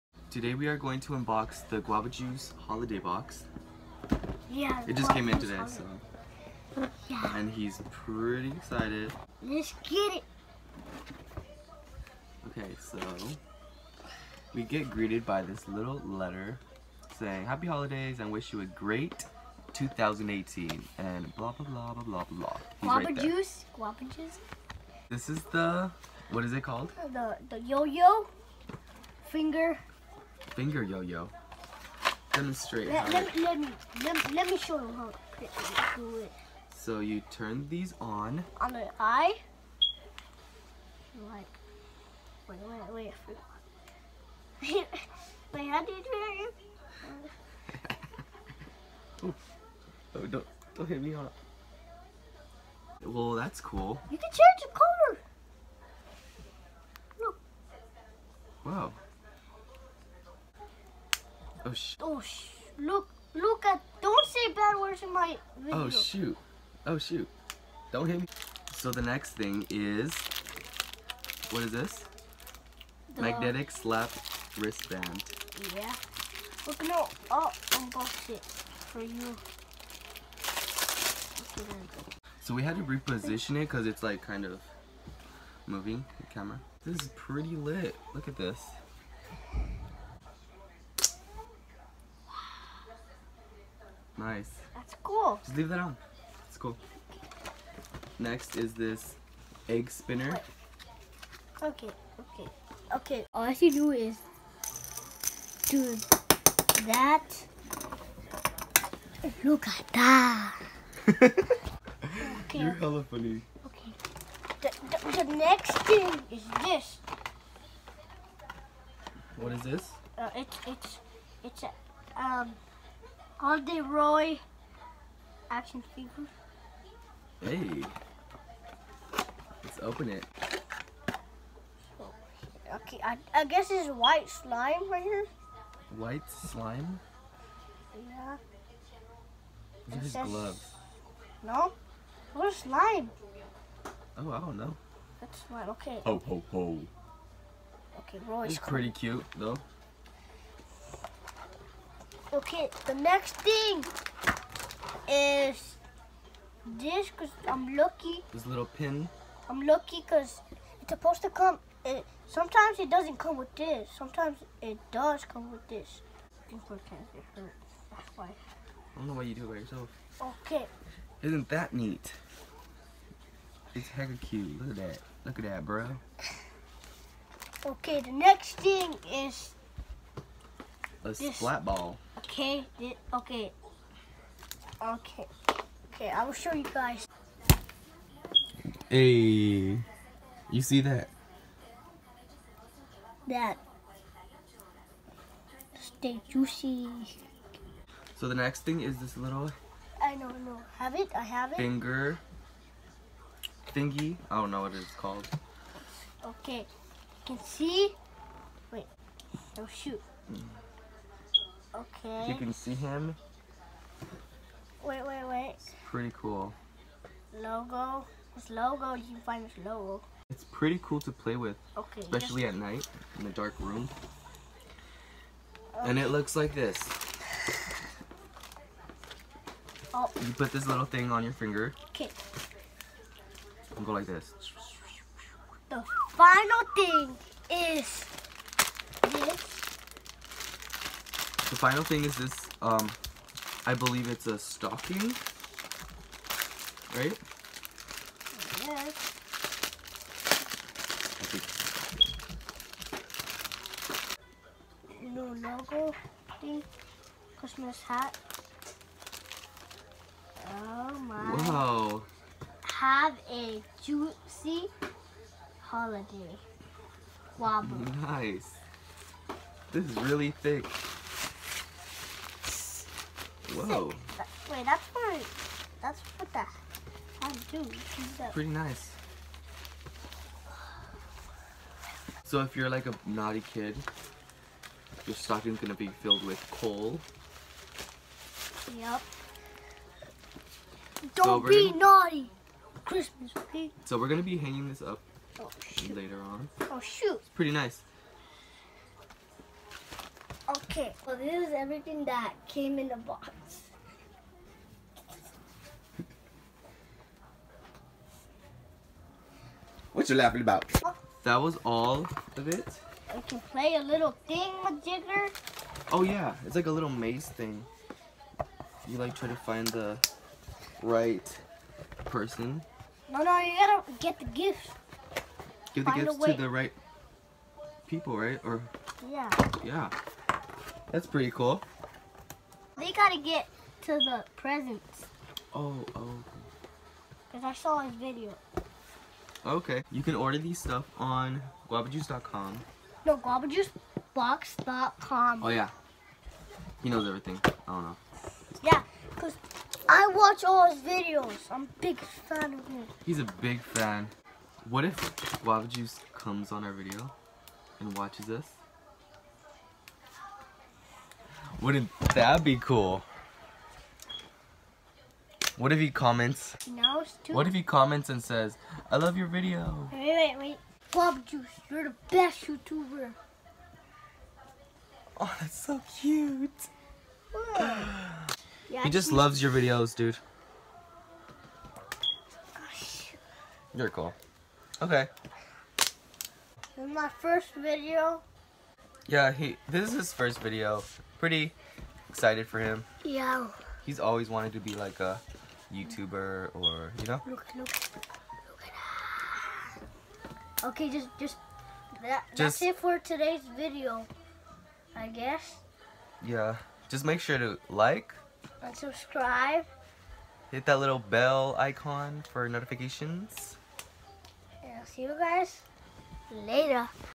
GUAVA JUICE HOLIDAY BOX!! [Unboxing Video]
We received the brand new Guava Juice Holiday Box today and we were really excited to unbox it! My favorite item was the Green Guava Splat Ball! All of the toys are really fun to play with.  I cannot wait to purchase more Guava Juice Boxes in the future.
I’m really excited to finally have my first YouTube video up on my channel! Can’t wait to create more content.